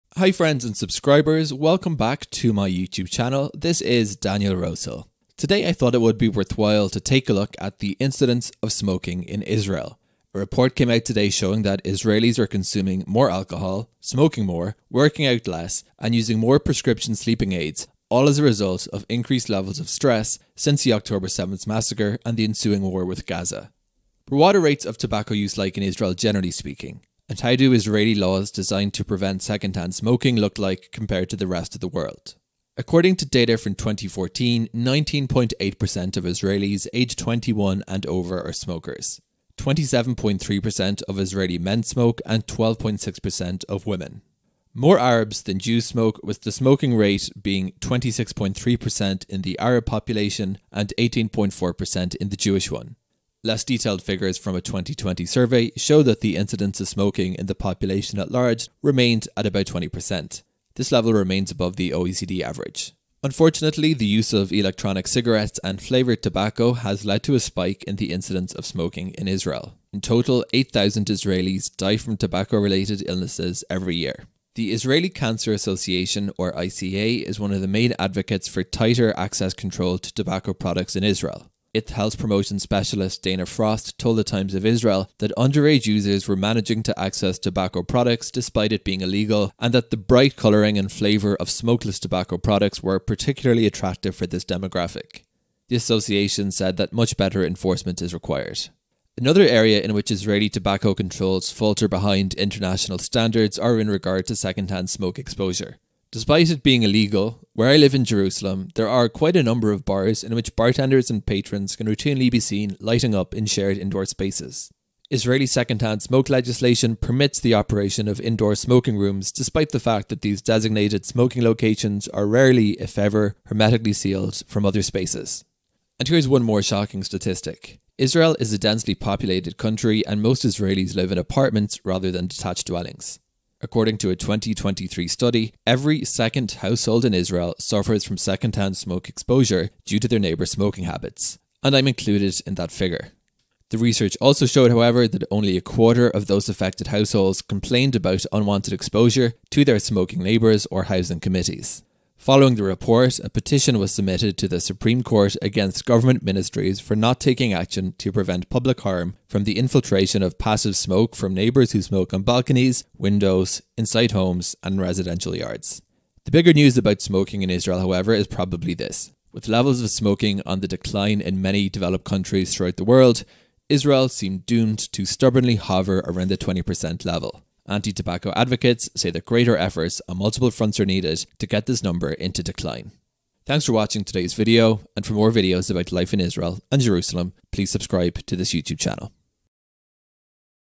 20% Of Israelis Are Smokers - Tobacco Use In Israel
According to repeated studies approximately 20% of Israel's adult population are regular smokers - and rates of smoking, alcohol use, and sleeping aid consumption are all on the rise due to elevated levels of stress and anxiety associated with Israel's ongoing war against militant factions in Gaza.

This video looks at trends of tobacco use in Israel as well as the problem of secondhand smoke exposure with every second household in Israel suffering in silence as a result of their neighbors' smoking habits.